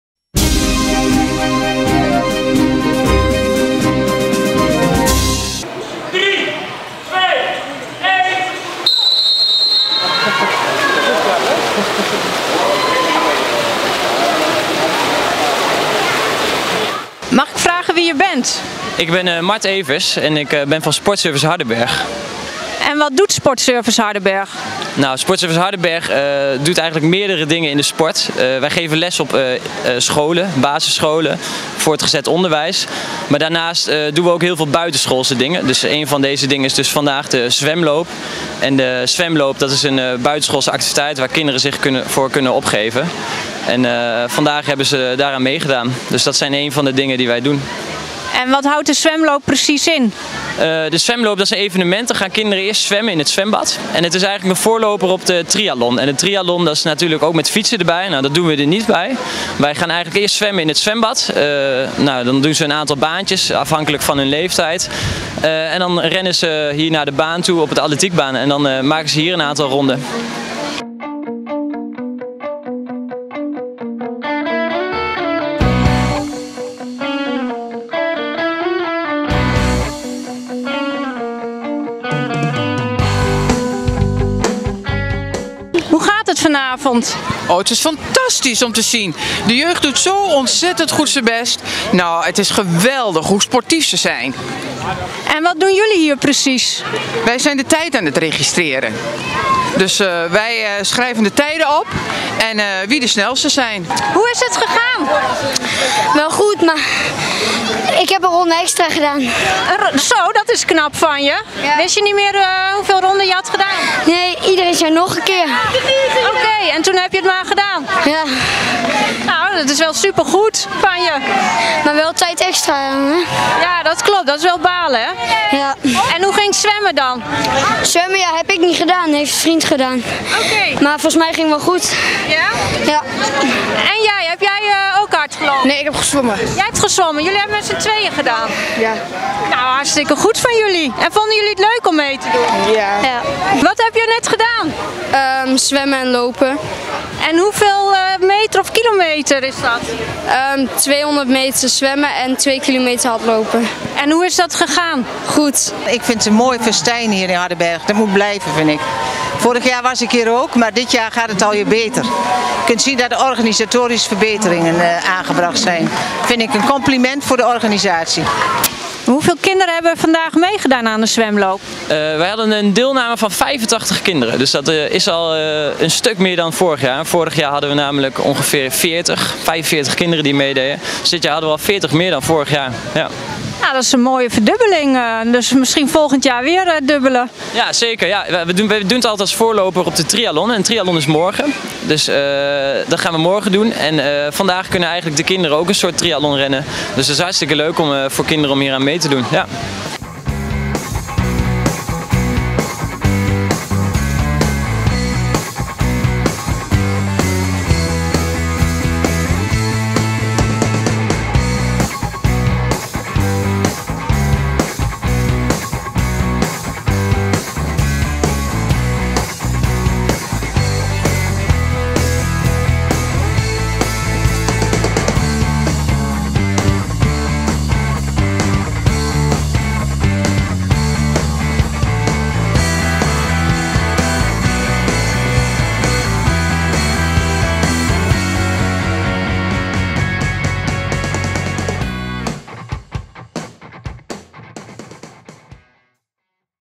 Zwemloop 2017 Hardenberg
Als voorloper op de triathlon in Hardenberg wordt de zwemloop voor kinderen georganiseerd.